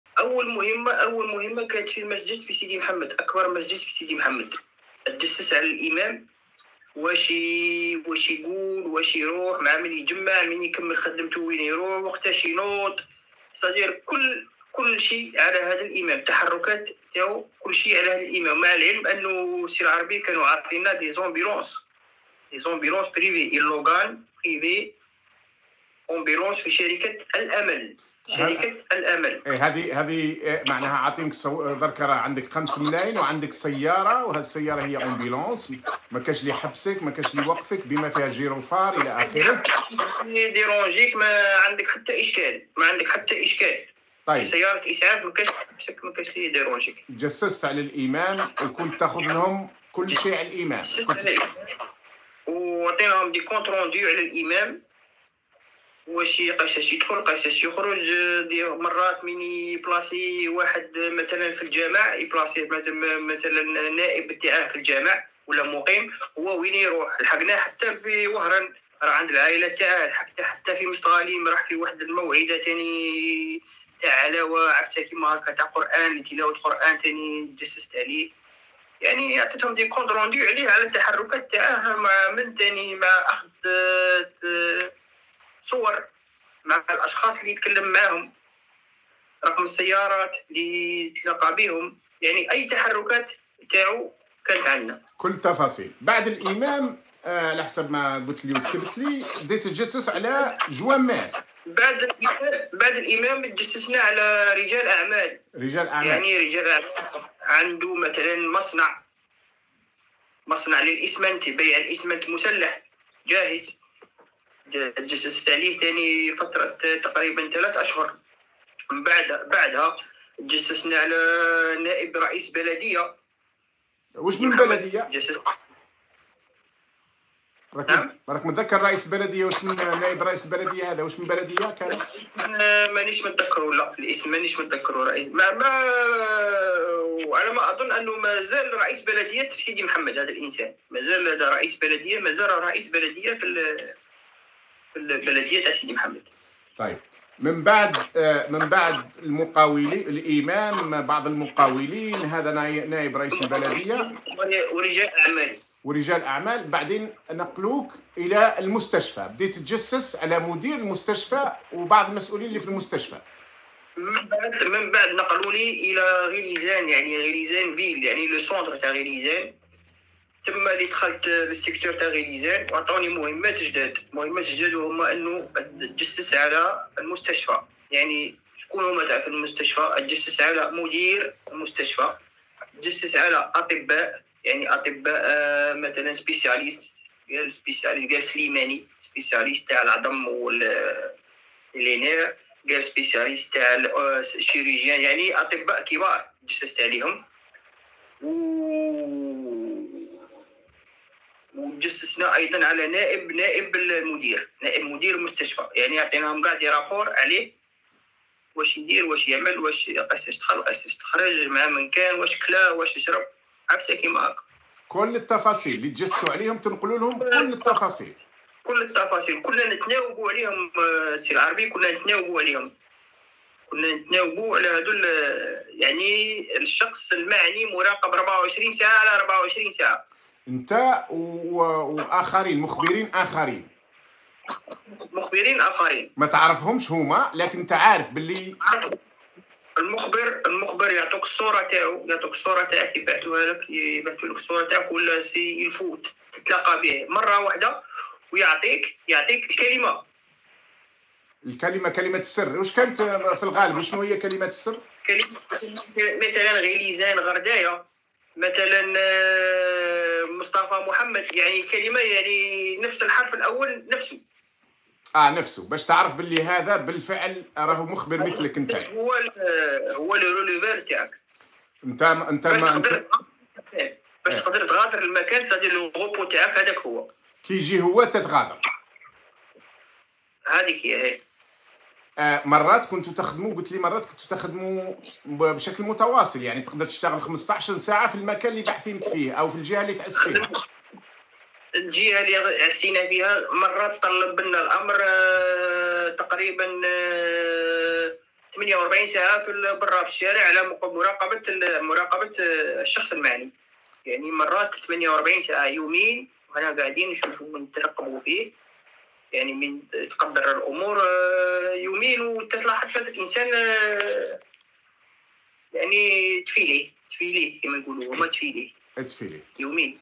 أول مهام المخبر الهارب "منير"...التجسس على إمام ورجال أعمال ومسؤولين مدنيين !
محمد العربي زيتوت 02 أكتوبر 2022